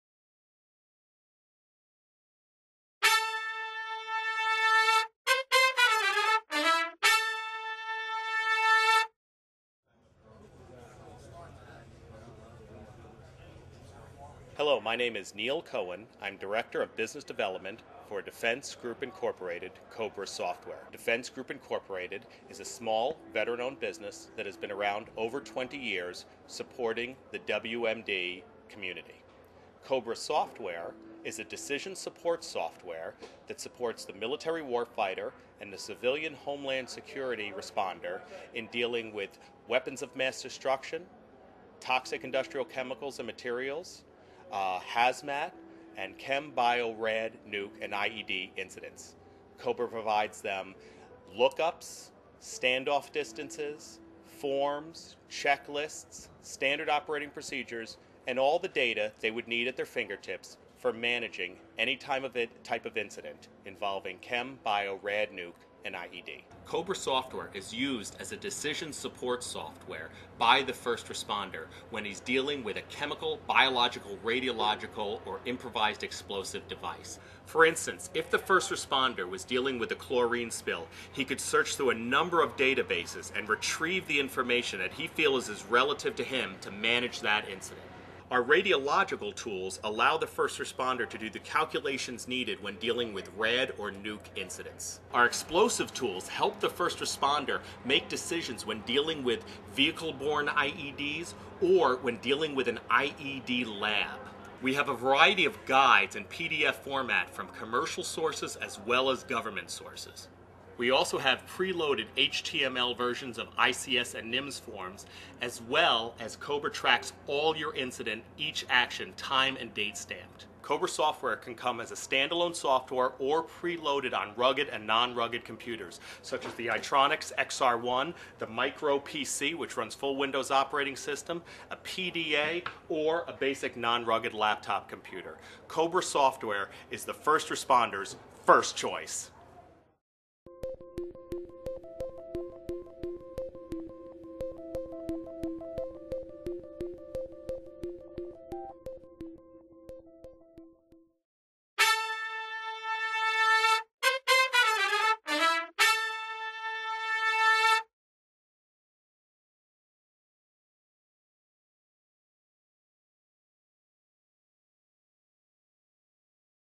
CoBRA overview interview from GSN
Overview of CoBRA software, all hazards decision support tool for First Responders (Video from 2010, later versions of software released)